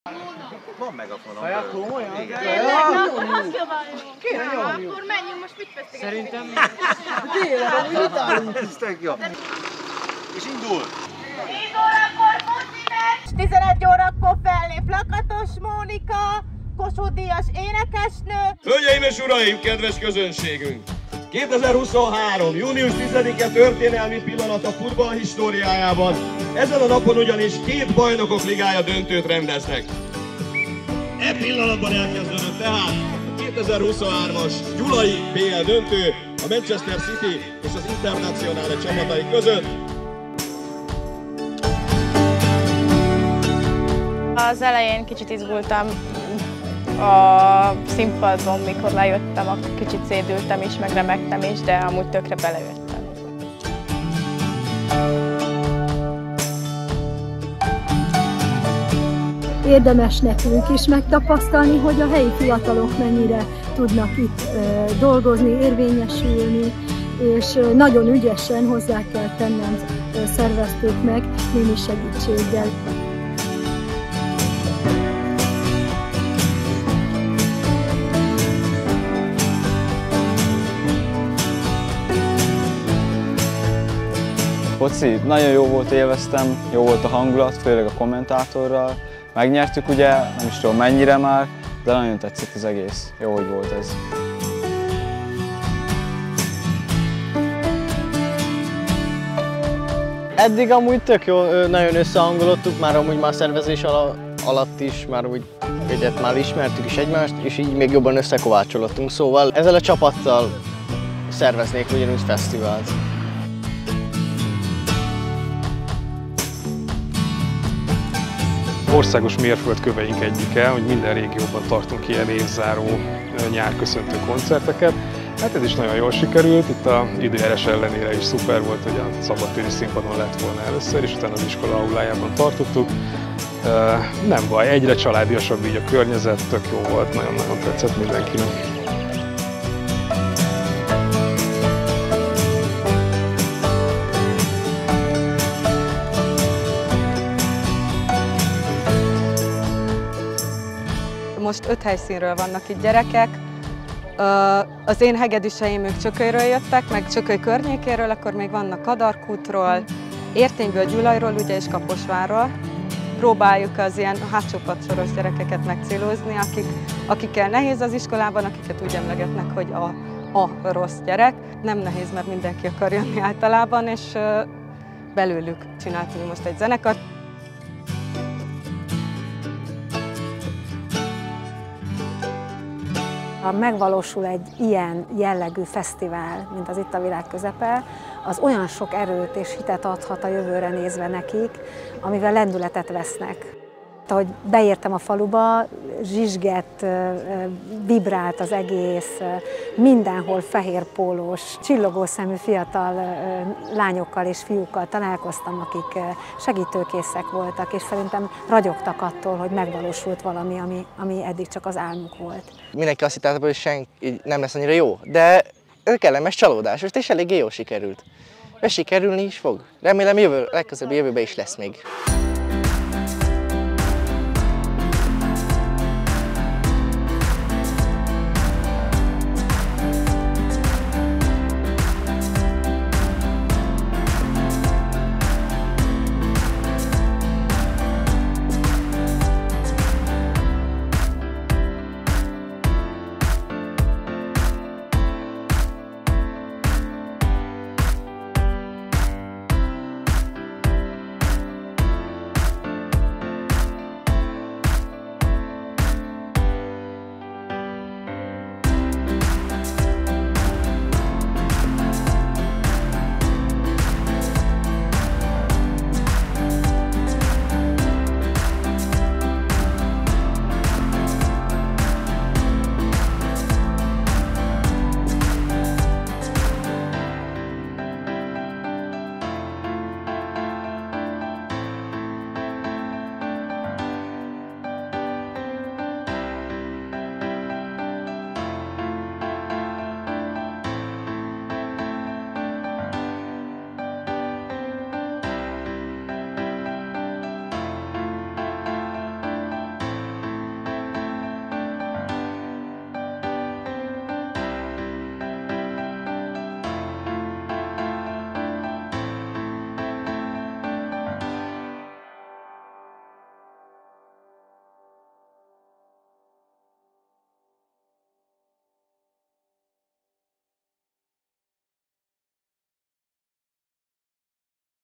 Itt a világ közepe! – fesztiválösszefoglaló
Miklósa Erika koncertjével zárult Gyulaj első összművészeti fesztiválja, az Itt a világ közepe! amely a helyi fiatalok számára szervezett médiafoglalkozások záróeseményeként jött létre.

Az egész napos eseménysorozat a Magyar Máltai Szeretetszolgálat égisze alatt a Szívlapát Alapítvány által szervezett oktatási projekt célállomása volt. A helyi fiatalok lelkes csapata professzionális segítséggel, de önállóan valósította meg a hagyományteremtő szándékkal született fesztivált. Bővebben